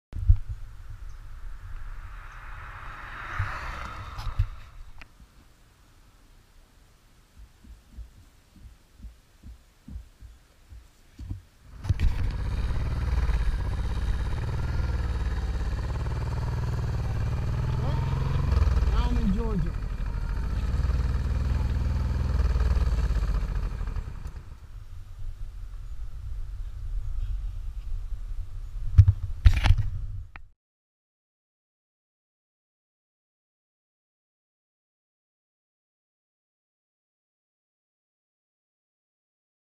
North Carolina/Georgia State Line
The funny thing which I didn't show, I stopped to buy supplies for the night, which included beer. They said it was a dry county. I asked how far the next county was, and they said about 200 yards, which was now another state. I bought my beer and I'm happy.